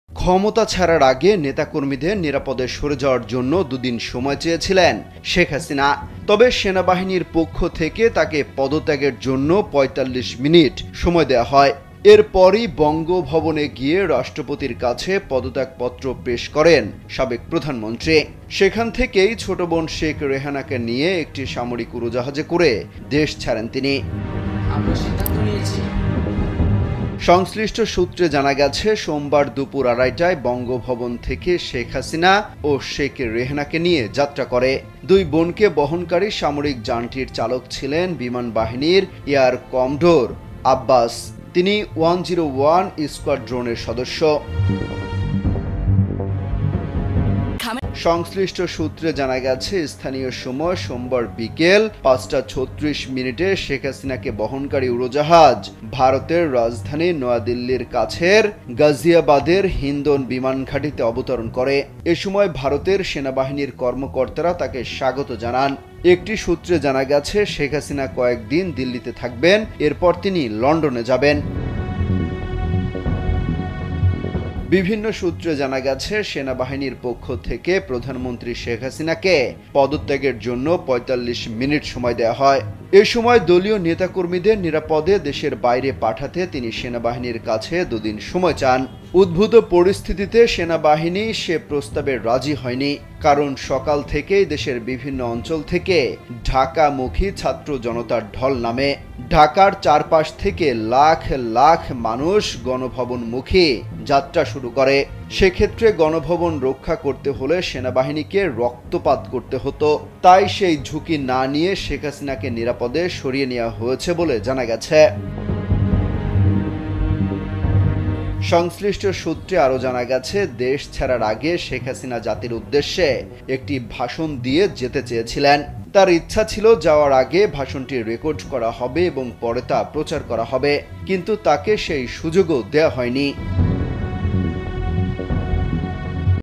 ভাষণের জন্য 10 মিনিট সময় চেয়েছিল কিন্তু 45 মিনিটের মধ্যে পালিয়ে যেতে বাধ্য করেছে সেনাবাহিনী
কোটা আন্দোলন ভয়াবহ ছাএদের জয় হউক |মানবতার ফেরিওয়ালা আজ কোথায় ছাএদের র*ক্ত জড়াচ্ছে কিছু করার নাই

Please save our Bangladeshi students